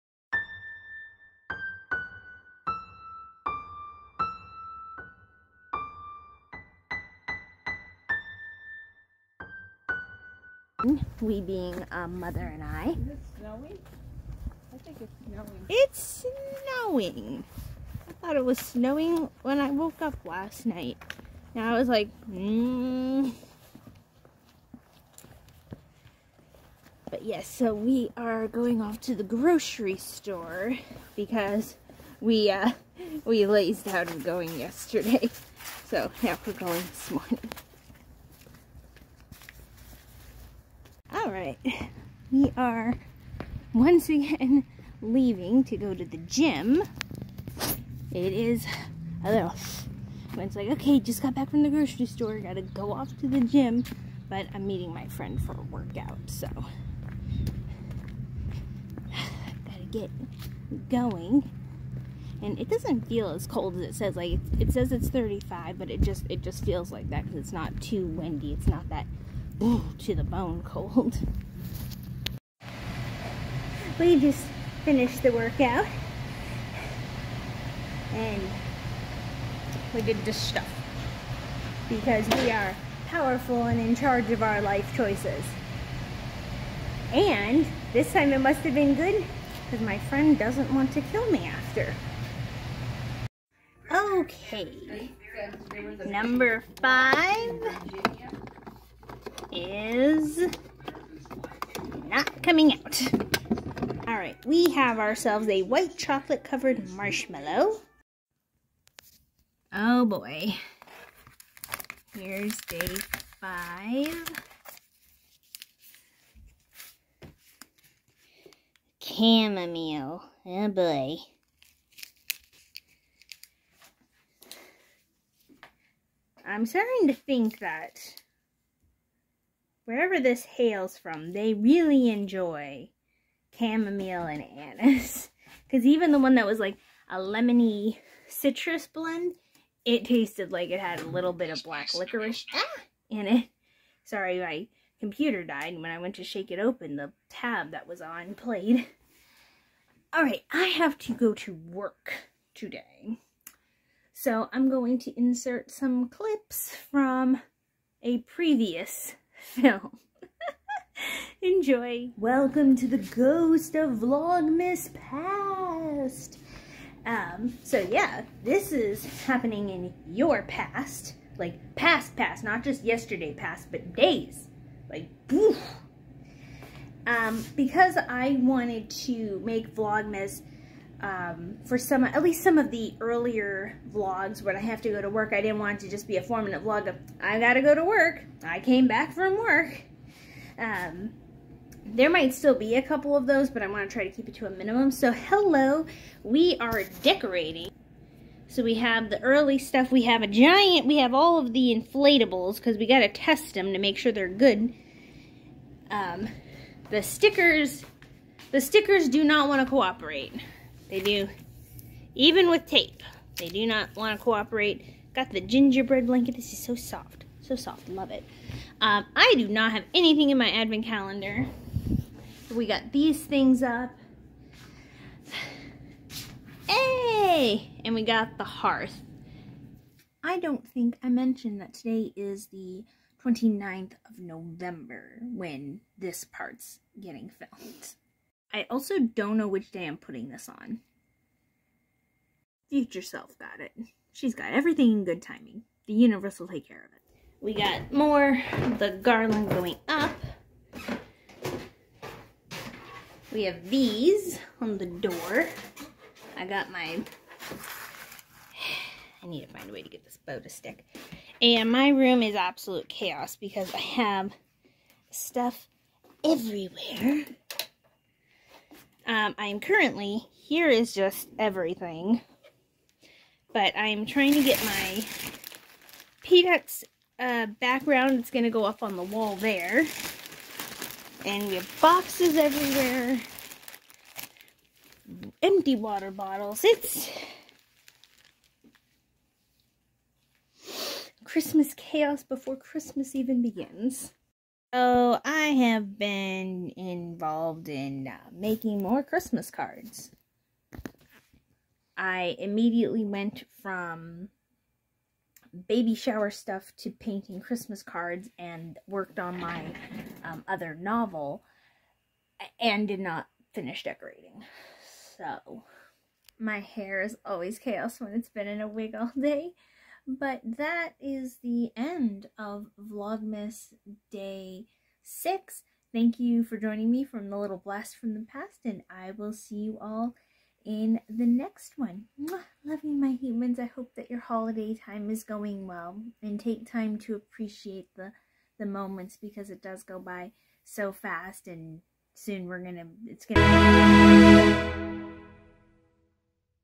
Vlogmas 2023 Day 5: Into VLOGMAS PAAAAST
I want to not have every single workday vlog be ridiculously short and boring so I pre-recorded some holiday prep to toss into at least one to spice it up. Enjoy..my hair in the least should give a giggle. ~FCA If I say '6' in the video, I looked at the date post midnight. Apologies.!FCA